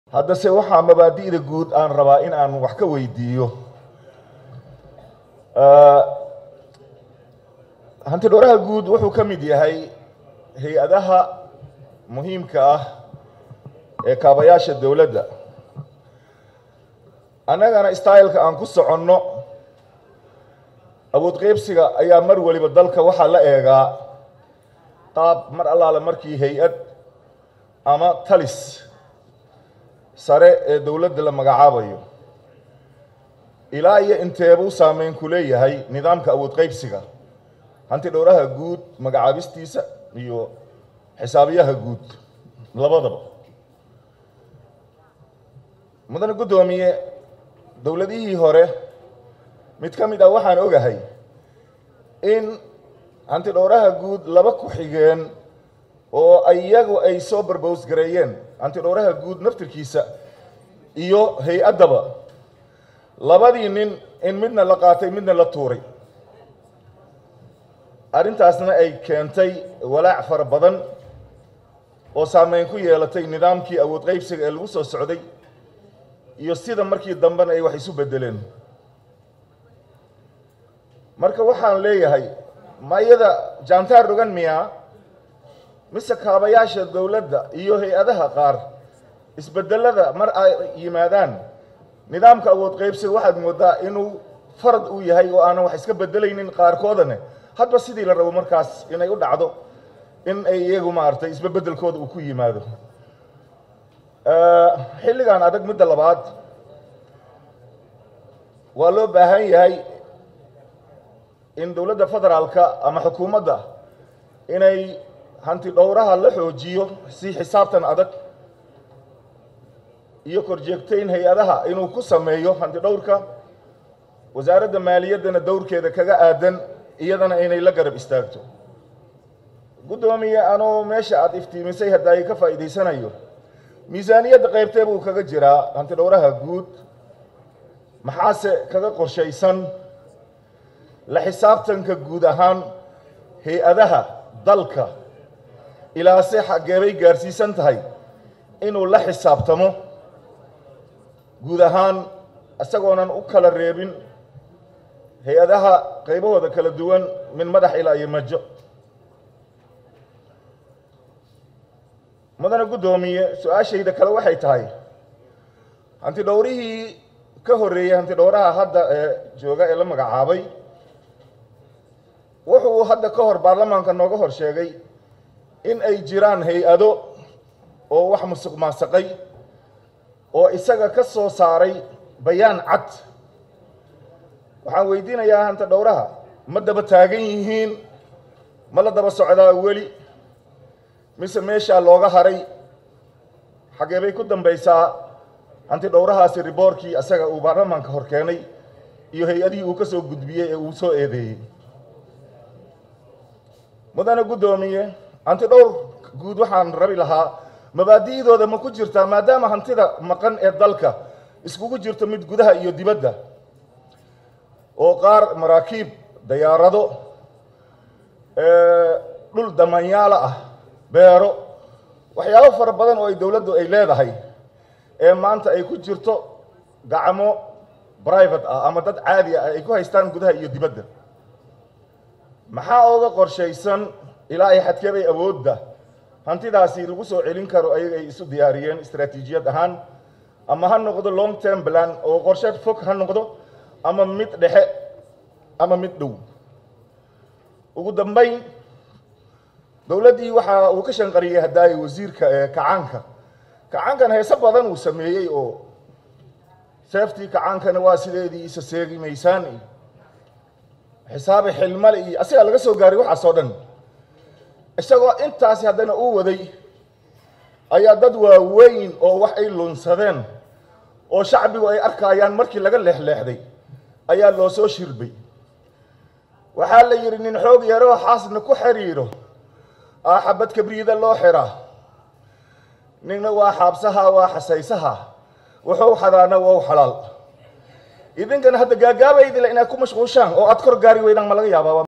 Maxamed Cumar Dalxa : ''Nin xoog yaroo xaasna ku xariiro ayaa xabad kabriid ah loo xiraa ,
Maxamed Cumar Dalxa : ''Nin xoog yaroo xaasna ku xariiro ayaa xabad kabriid ah loo xiraa , Nina waa xaabsahaa wuu xaseysanaa Wuxu xadaana waa u xalaal amee sheekadu hanti dhowrka noqota.''